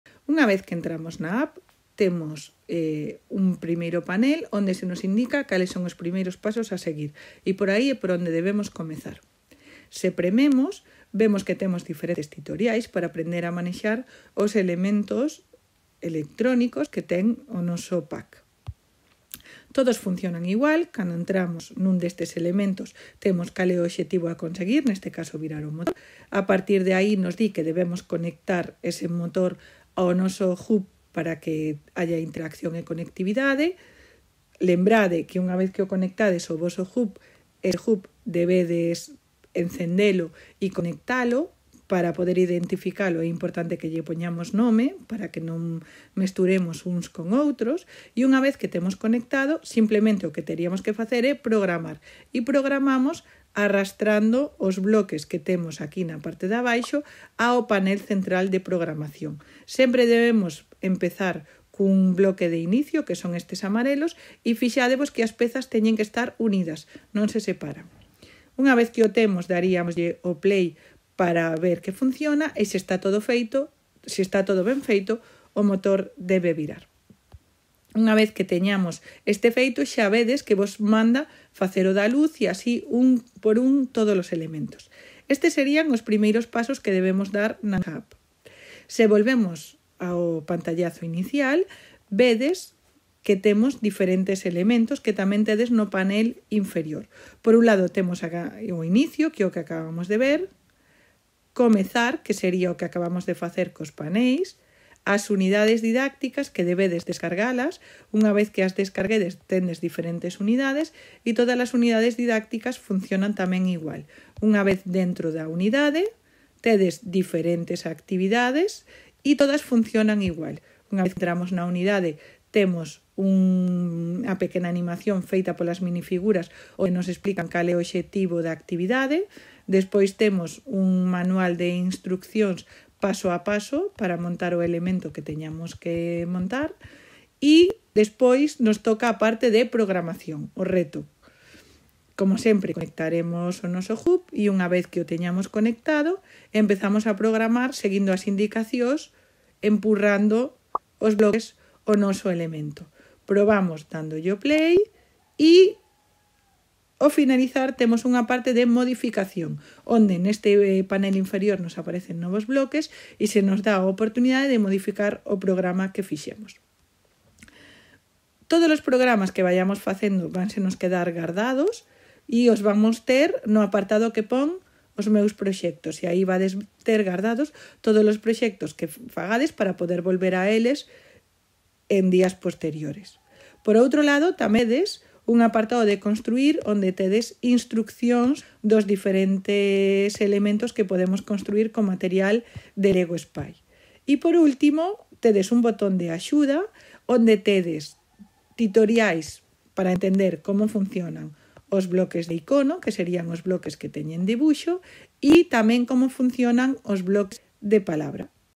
Achegamento a Lego Spike
Vídeo titorial con instrucións básicas para entender o funcionamento do software Lego Spike.
Material elaborado nunha licenza por formación da Consellería de Cultura, Educación, Formación Profesional e Universidades da Xunta de Galicia no curso 2023/24.
Autoría:  Verónica Camiña García e Miguel Otero Bernárdez